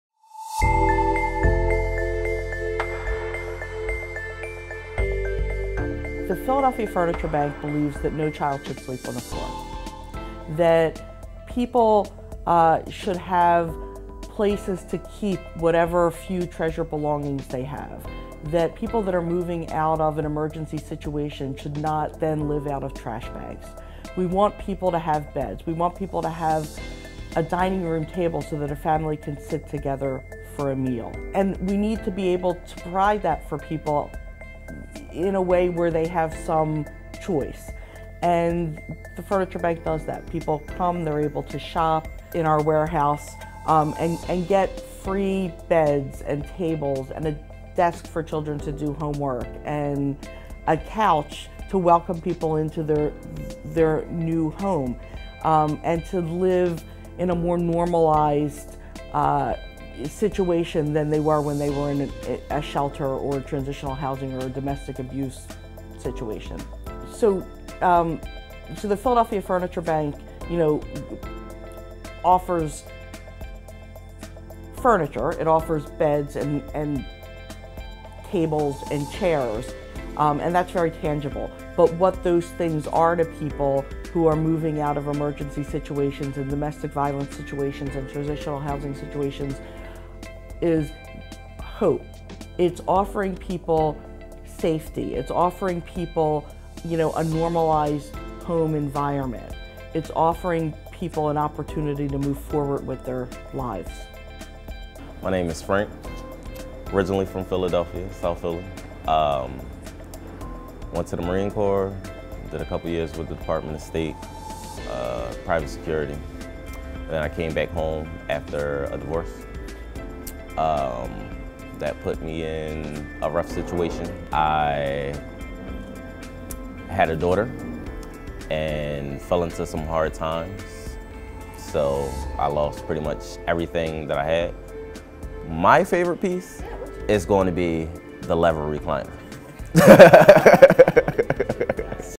The Philadelphia Furniture Bank, another word for HOPE
Learn about what the Philadelphia Furniture Bank is, and how the Chair Affair will help to move the mission of no child sleeps on the floor a reality.
The Philadelphia Furniture Bank is the only one of its kind in the city of Philadelphia  We work with member agencies whose clients are coming out of homeless situations, shelters, and other hardship issues.
Listen to stories and find out what makes us so special.